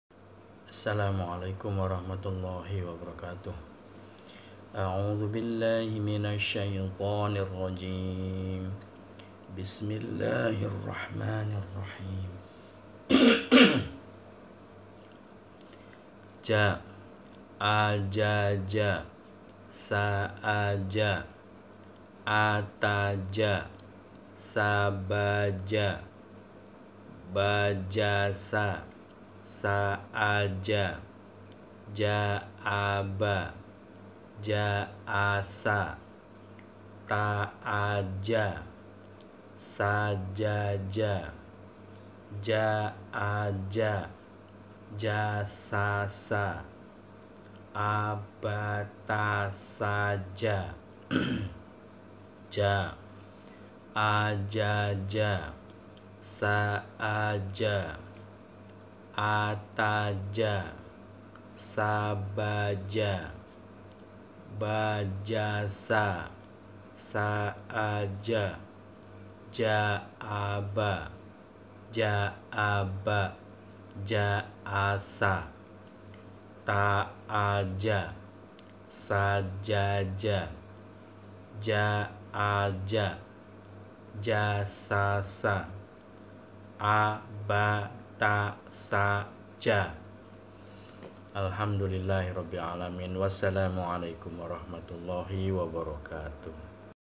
IQRO 1 PAGE 4
Easy learning to read th Holy Quran with the Iqro Book.